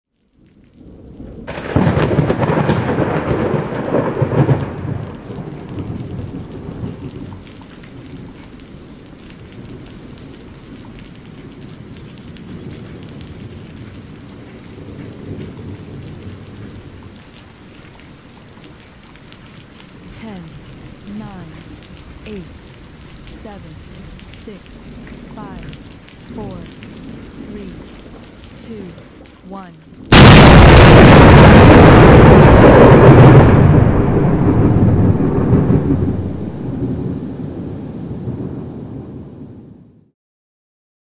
30 Second Rain Timer
This is a 30 Second rain themed countdown timer